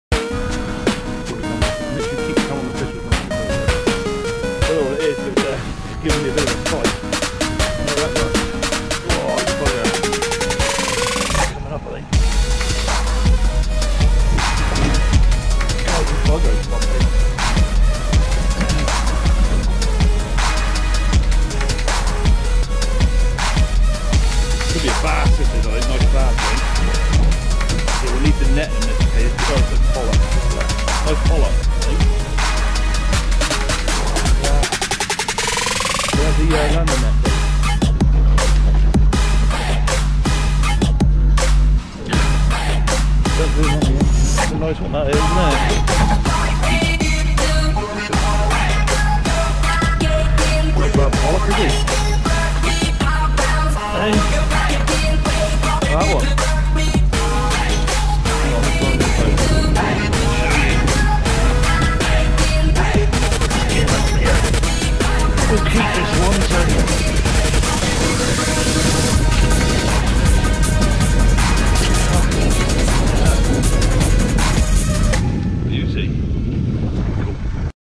pollack fishing padstow
nice size pollack caught on the boat padstow cornwall